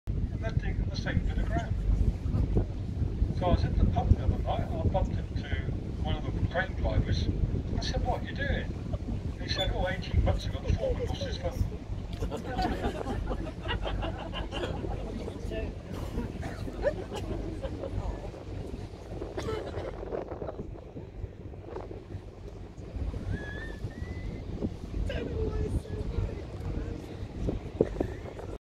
Dartmouth Cruise Devon , Cardiff Castle
Cruise Captain tells another joke